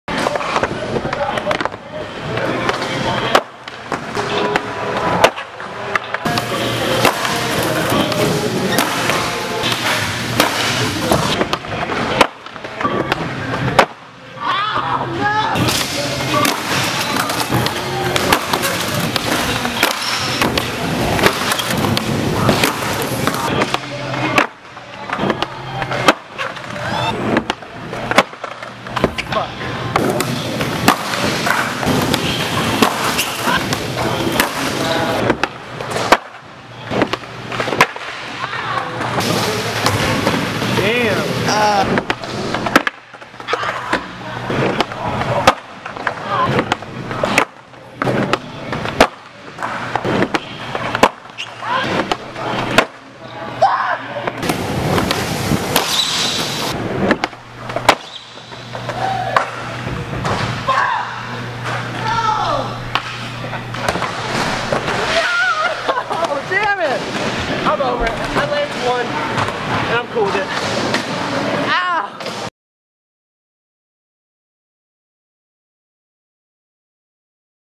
Late Back Foot Hardflip 8 Stair
A fuck ton of robbed attempts at late back foot hardflipping the 8 stair at Midtown. This was my first time back on a board in 5 weeks and I could NOT roll away from this trick. I wound up completely exploding one of my bearings and giving up for the night. I'll be back to land it and get it better soon!